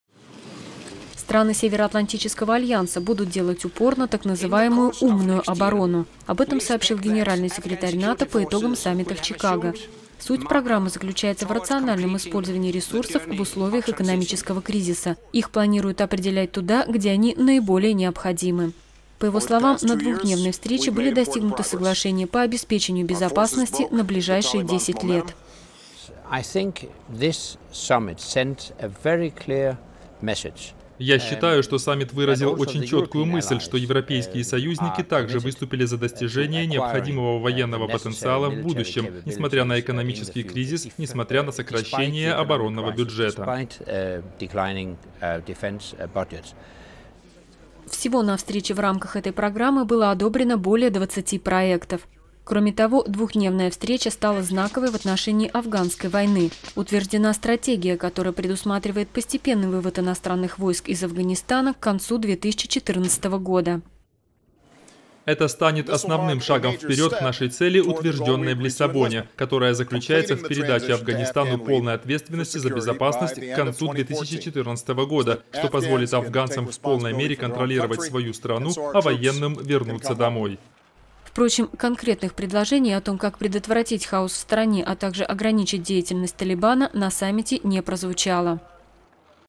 НАТО делает упор на «умную оборону»
) В Чикаго завершил работу саммит НАТО. По итогам встречи было утверждено несколько десятков межнациональных проектов. Главный упор делается на «умную оборону».